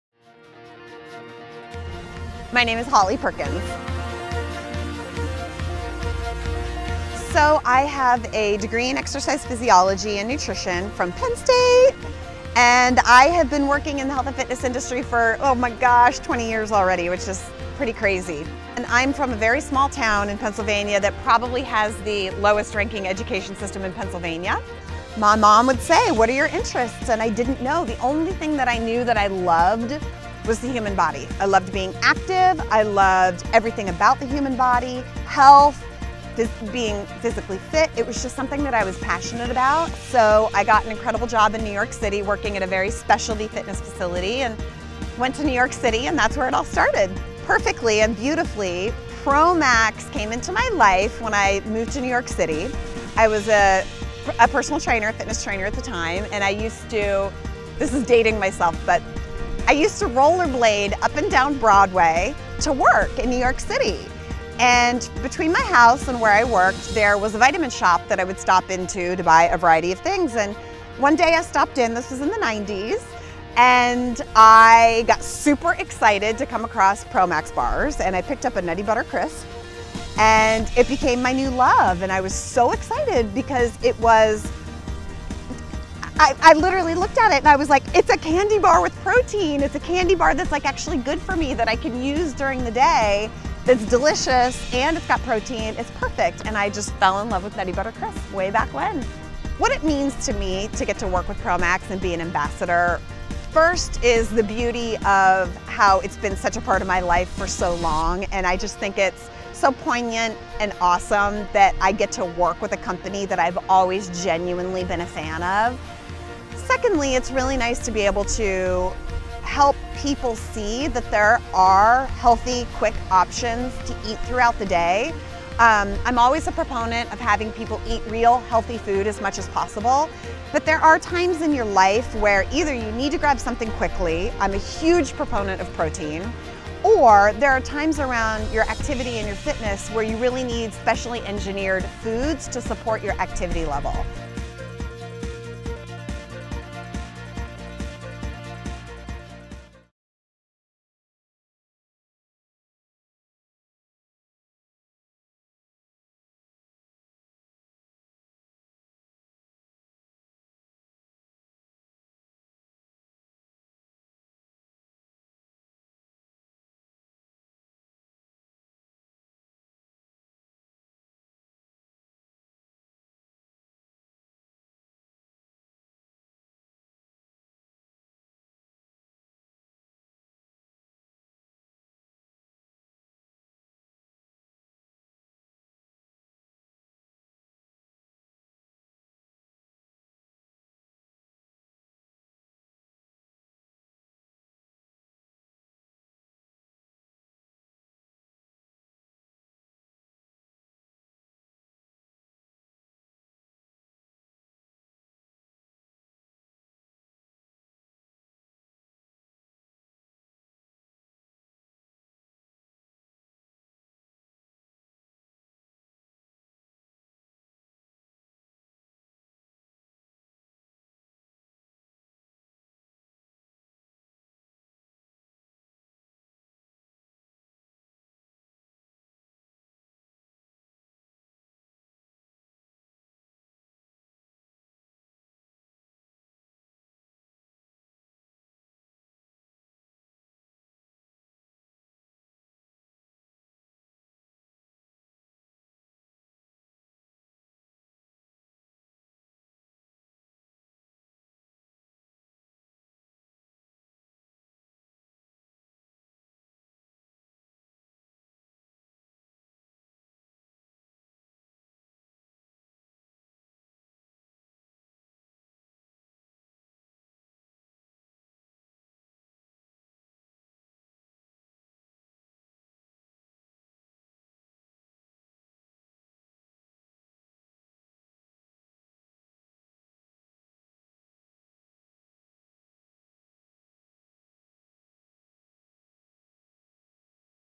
Meet Promax Brand Ambassador, Holly Perkins!
Holly does it all and she's our Ambassador! Learn more about who she is, her story and why she LOVES Promax.